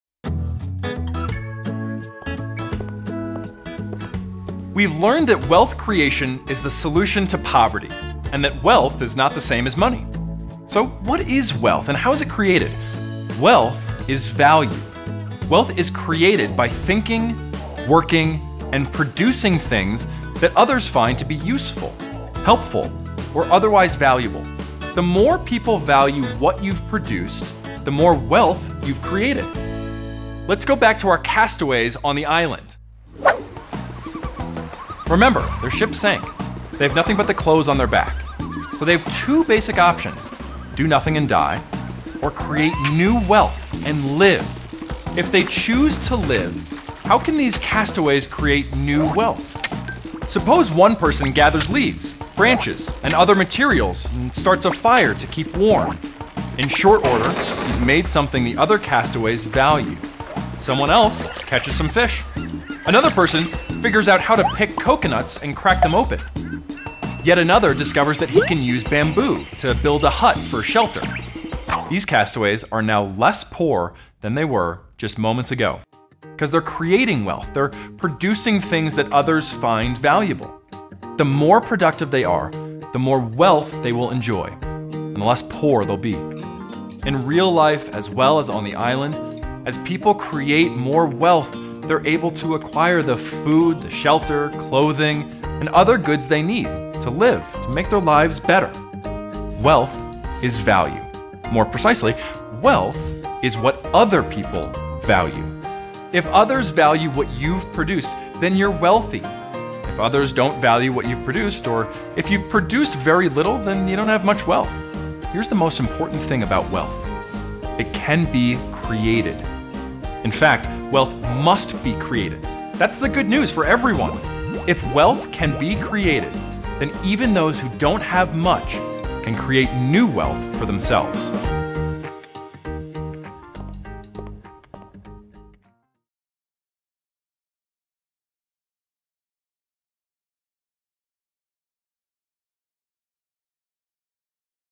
Wealth Creation III: What Is Wealth?
Wealth is anything other people value. Creating wealth requires thinking less about yourself and more about others because creating wealth for yourself means producing something others want, need, appreciate, or otherwise value.